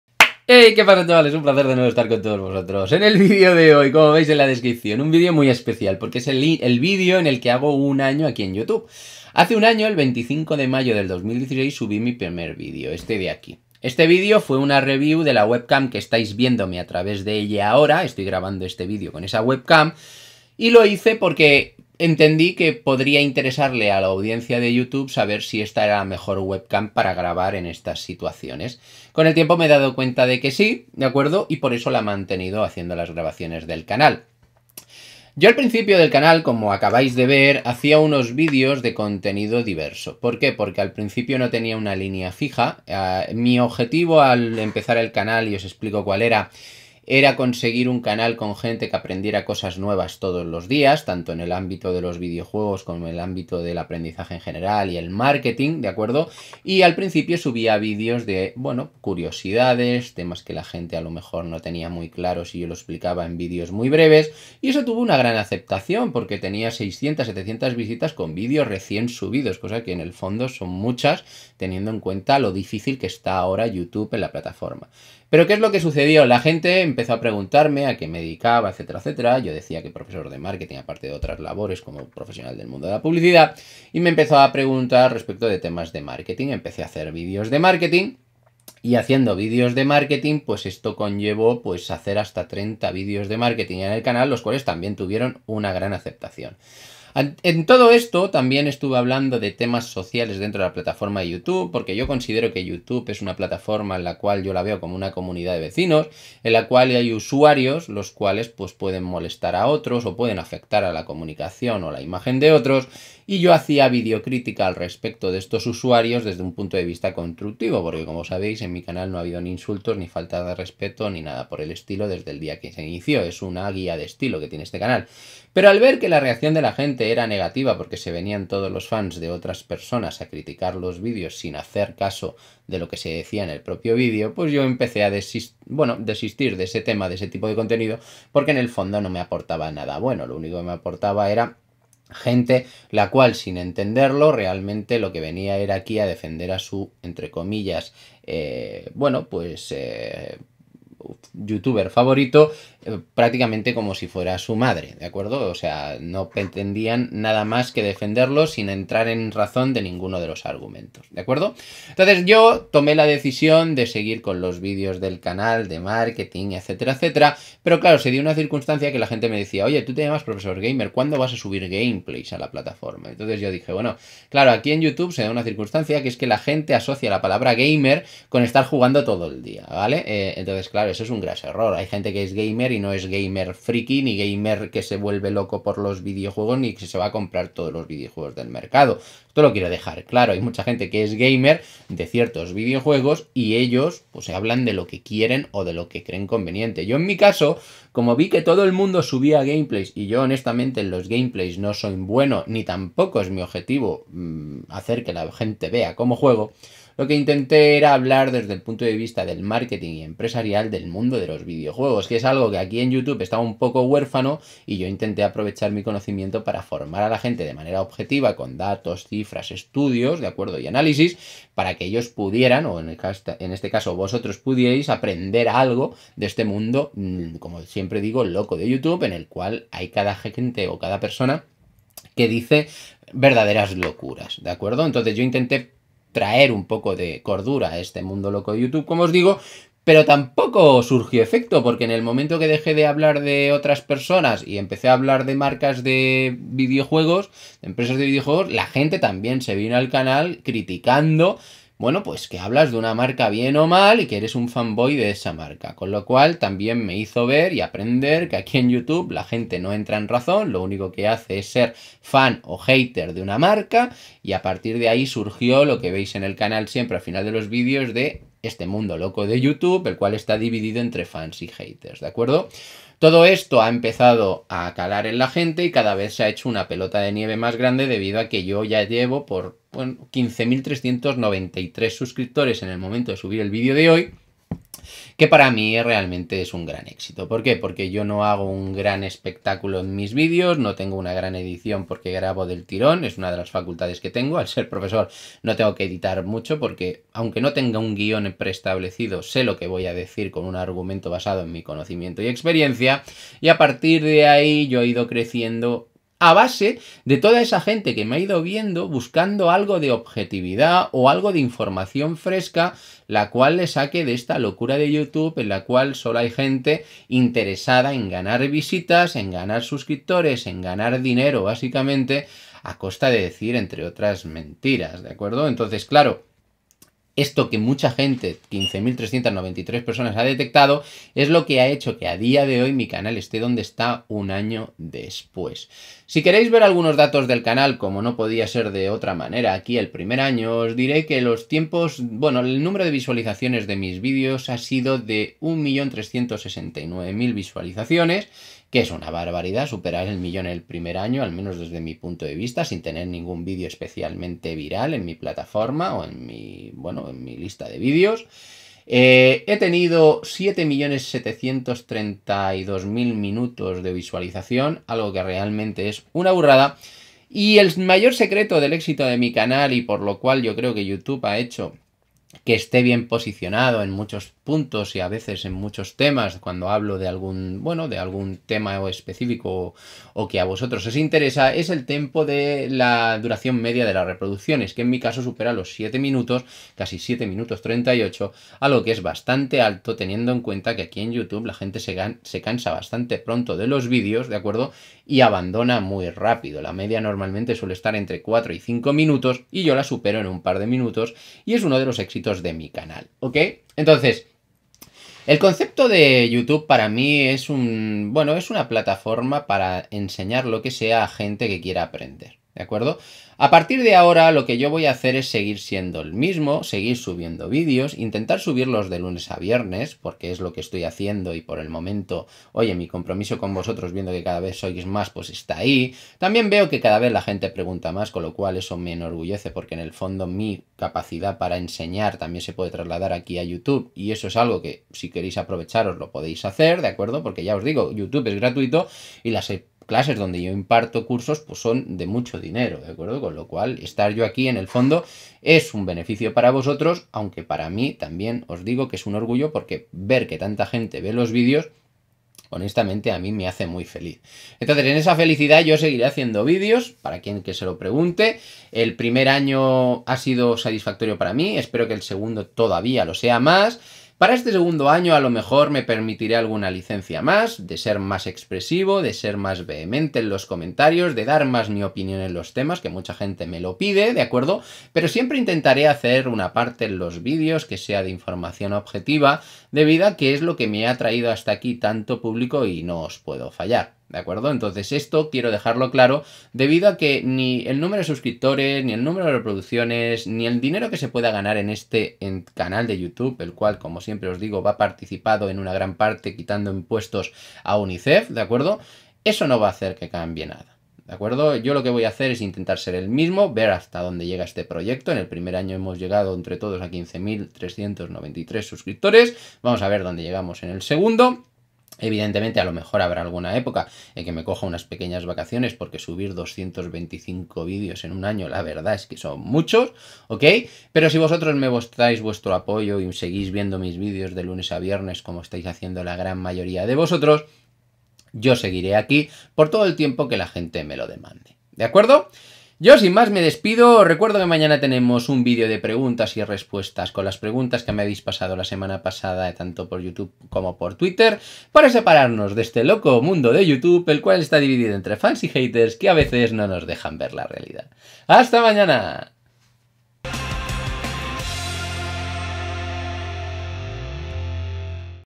1 AÑO EN YOUTUBE 🎂
Os cuento mi experiencia de mi primer año en YouTube.